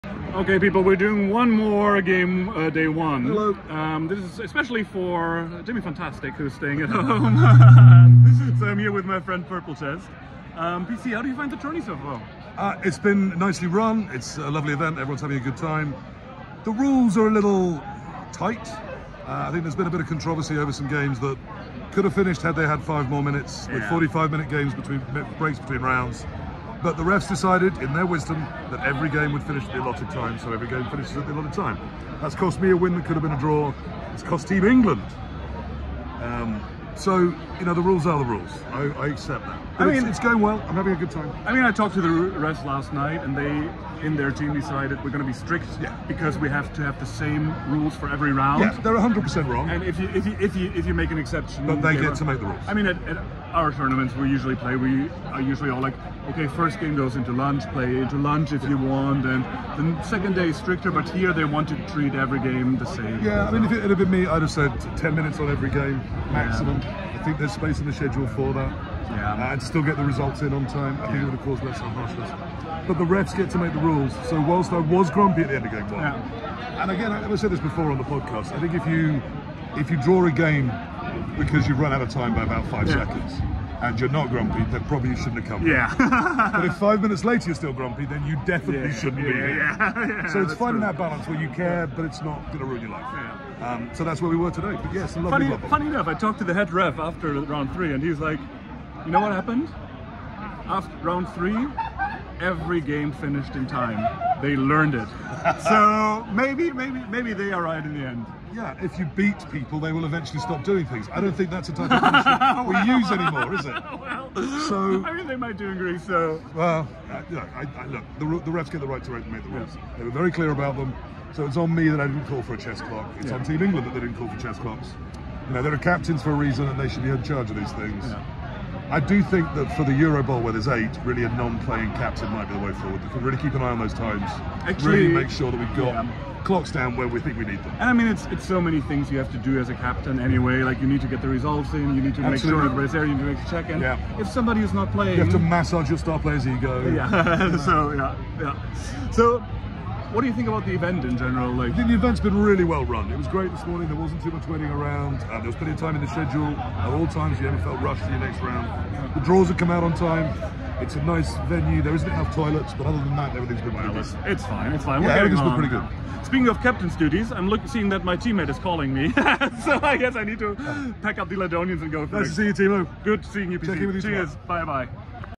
Volk chatting to PC after round 3 of Eurobowl '24
Vlogs from Eurobowl '24 - Greece!